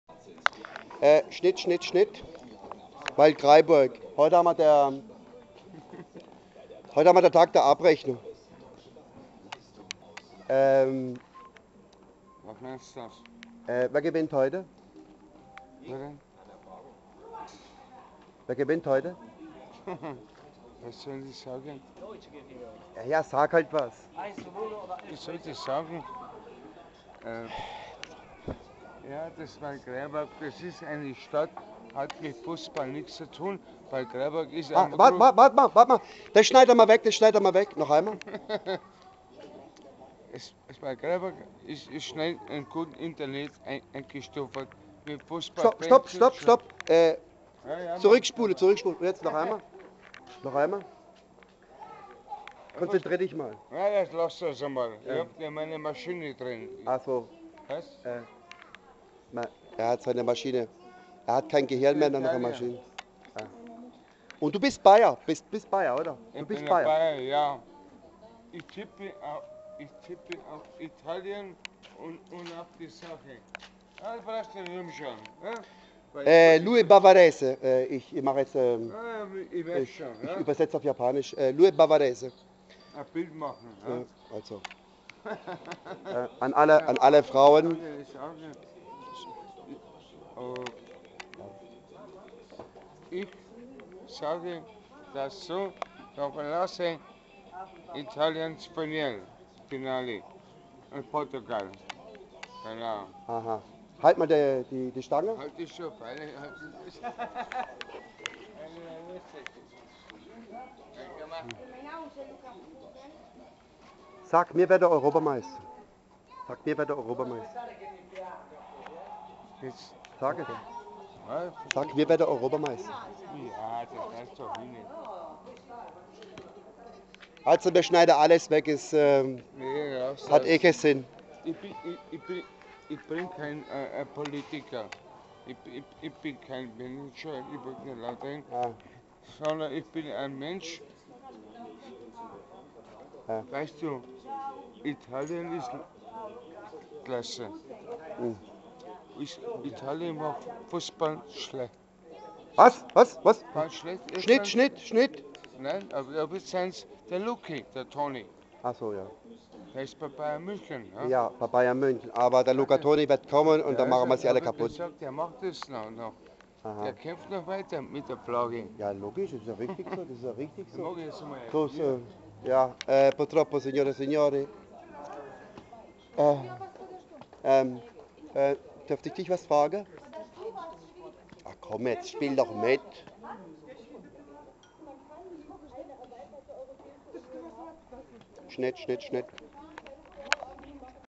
Ein Bayer in Waldkraiburg
er hat sein gehirn versoffen-er kann nicht mehr stehen-sein leben hat keinen sinn mehr-der einzige ausweg...innkanal...seine besten freunde/bier,schnaps,hirnlossigkeit,obdachlossigkeit,verdacht auf gehörlossigkeit,brunsblöd,